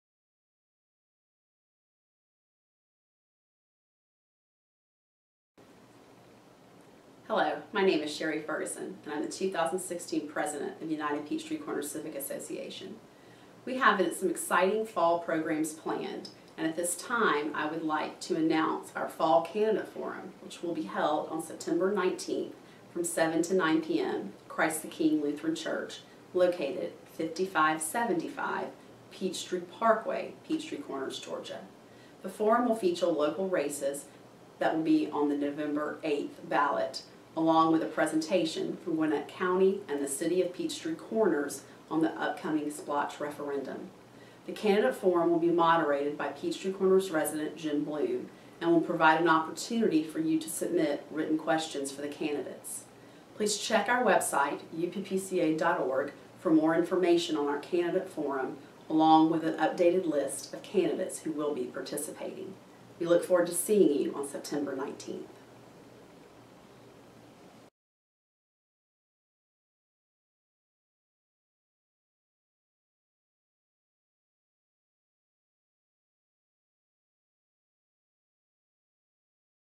UPCCA to Host 2016 Fall Candidate Forum - September 19, 2016
You're invited to the UPCCA fall candidate forum which will be held on September 19th from 7:00-9;00 pm at Christ the King Lutheran Church located at 5575 Peachtree Parkway, Peachtree Corners, GA.  The forum will feature local races that will be on the November 8th ballot along with a presentation from Gwinnett County and City of Peachtree Corners on the upcoming SPLOST referendum.  The candidate forum will be moderated by Peachtree Corners resident Jim Blum and will provide an opportunity for you to ask written questions of the candidates.